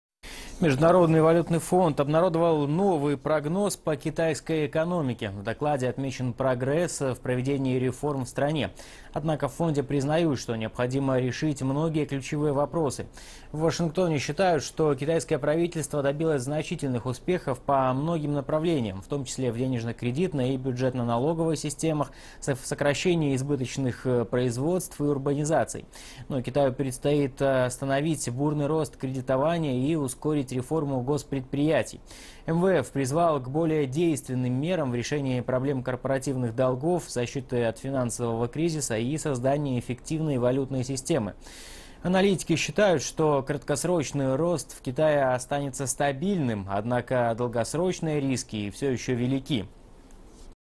МВФ отметил прогресс в реформах, но в КНР нужно решить ключевые проблемы
Китайская экономика